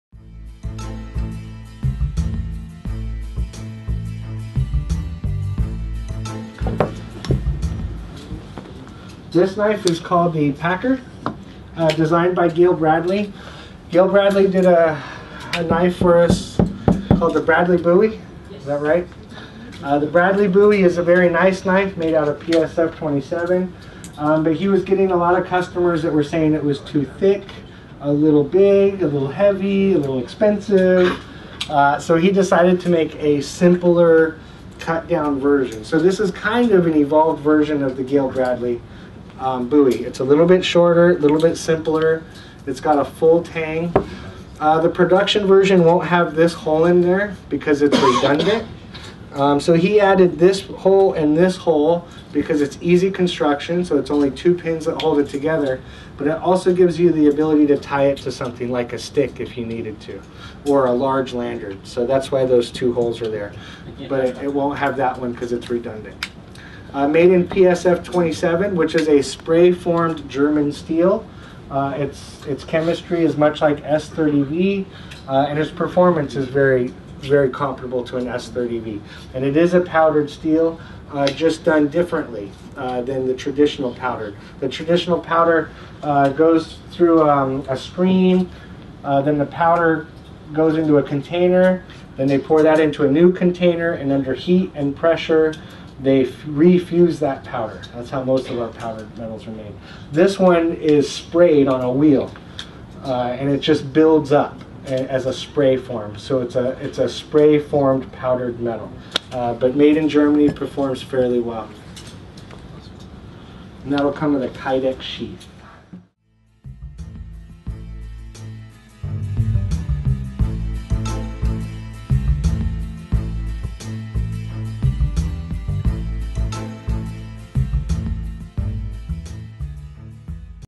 Spyderco 2016 Production Sample – Gayle Bradley Packer
For a more detailed look at the Spyderco Gayle Bradley Packer production sample, go

Last February, Spyderco organized the 12th annual ‘Amsterdam Meet’ in, you guessed it, Amsterdam the Netherlands. During the meet, Spyderco’s Eric Glesser presented many new concept models, prototypes and production samples that Spyderco is working on. Around 80 knifeknuts attended meet and they were asked for feedback and opinions on the new designs.

This year, we used a live videolink to a big screen, so the entire audience could get a closer look at the various details of the knife during Eric’s presentation. This clip is taken from that videolink. I hope you understand that this was not a purpose-made video presentation, but rather a taped portion of a live presentation. Nevertheless this video offers a nice first look at a new Spyderco design and its backstory.

Spyderco Forums, BladeForums.com, or USN as Mr Blonde